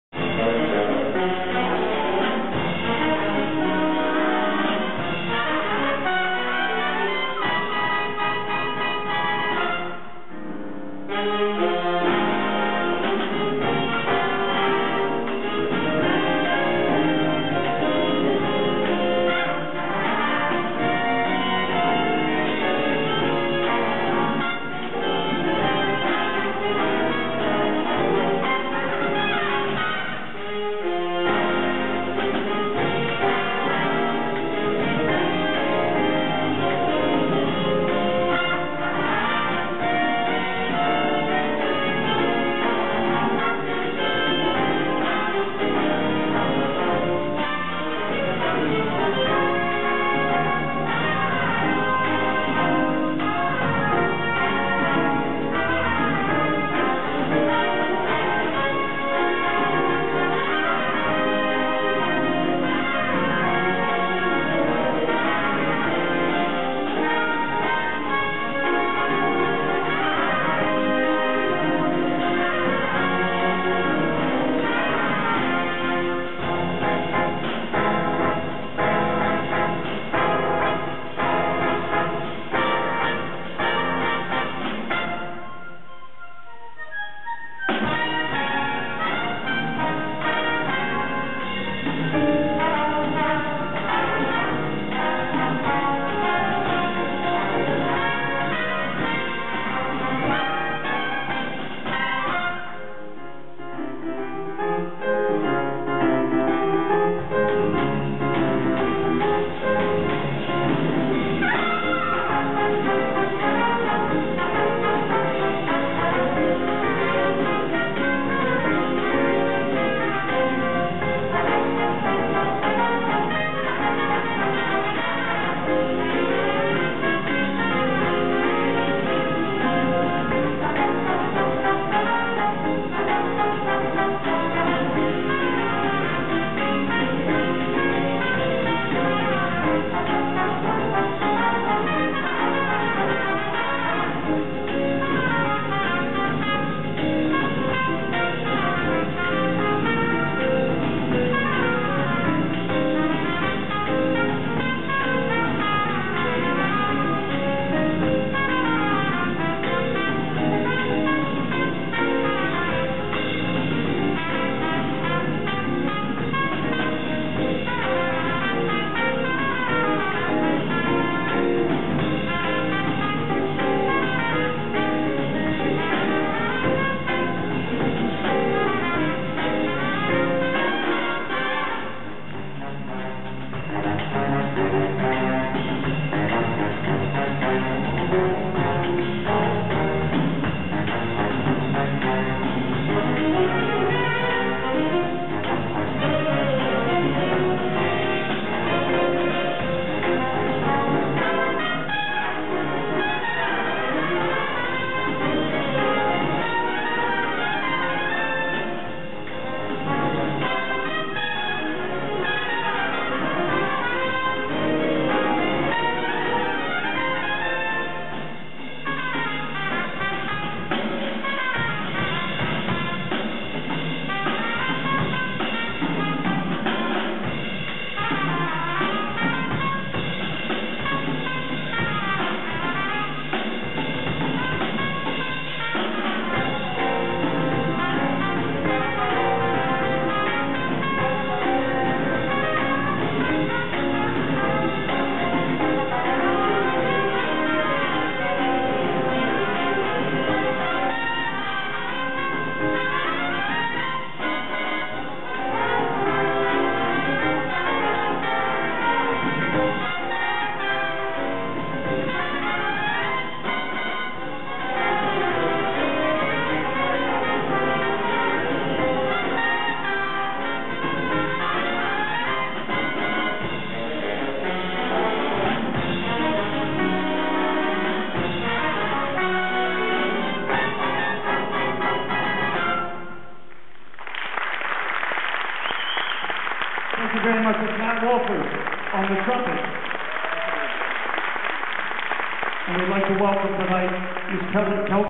Matty's Lehigh U. Jazz Ensemble
Matty is featured in this song from the winter concert of the Lehigh University Jazz Ensemble.